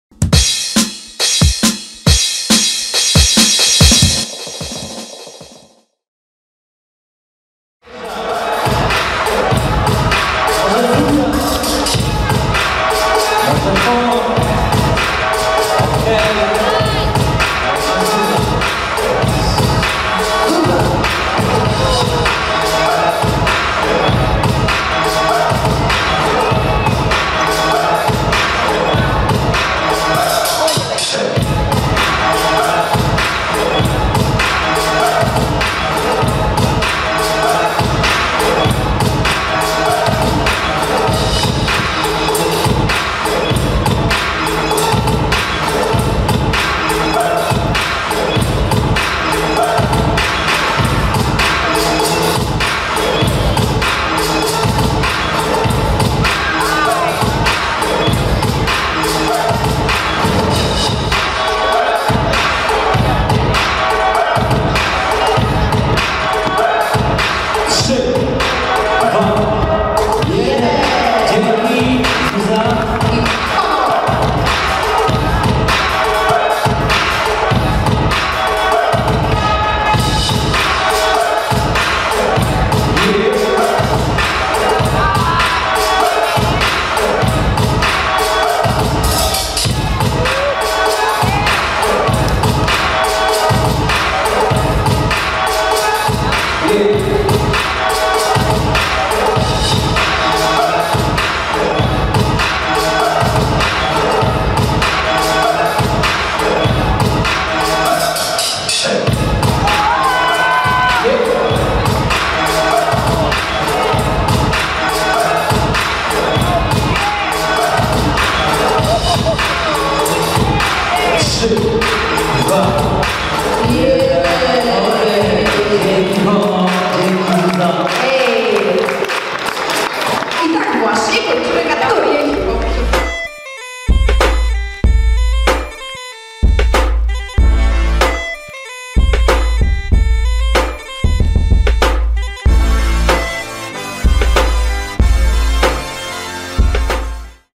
Ostrowski Styl 2018 / Ćwierćfinał Hip Hop 1vs1 / PoProstu Zuza vs Hono
Ostrowski Styl 2018 / Ćwierćfinał Hip Hop 1vs1 / PoProstu Zuza vs Hono / MOSiR Ostrów Mazowiecka / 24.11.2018.